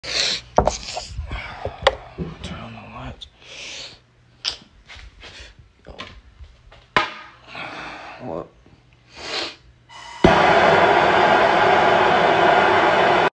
man really had to pee so bad
that it turned into a blow torch